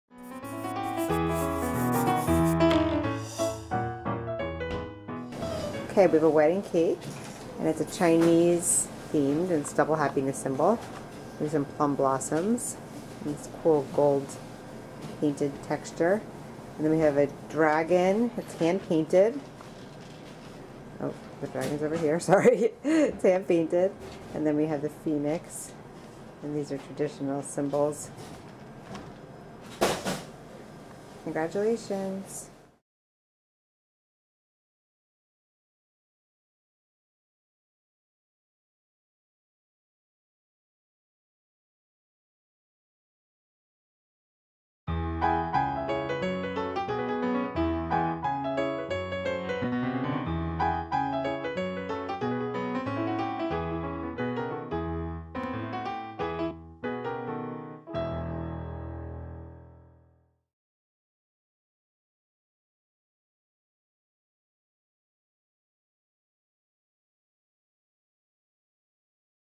Double Happiness Wedding Cake
We worked with the bride and groom to design this special wedding cake for their celebration at the Sunny Palace in East Brunswick, NJ.

Interesting in learning more about cake decorating?